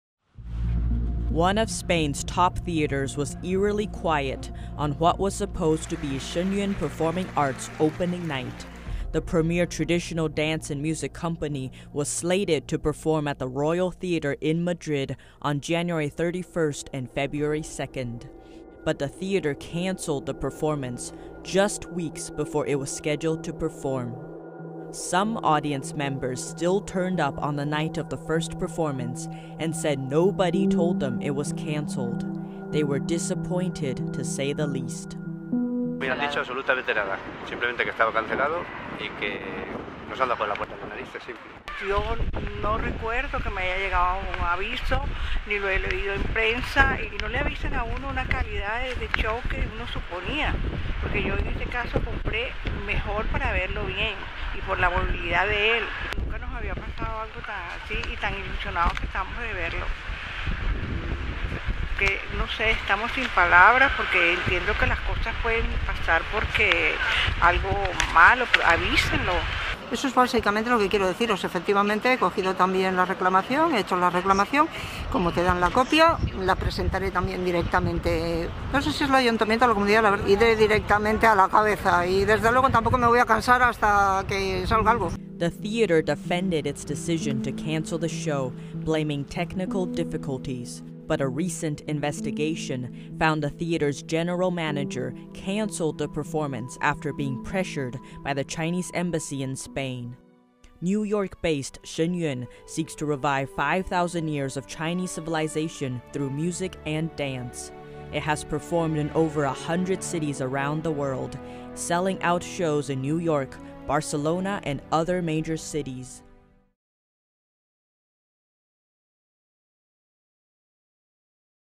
Madrileños rechazan la censura a los shows de Shen Yun en el Teatro Real
Se esperaba que uno de los mejores teatros de España estallara de vida y calor en la noche de apertura de Shen Yun Performing Arts el 31 de enero, en Madrid, pero en cambio la audiencia fue recibida con un vacío espeluznante debido a la injerencia de la embajada china para censurar el espectáculo.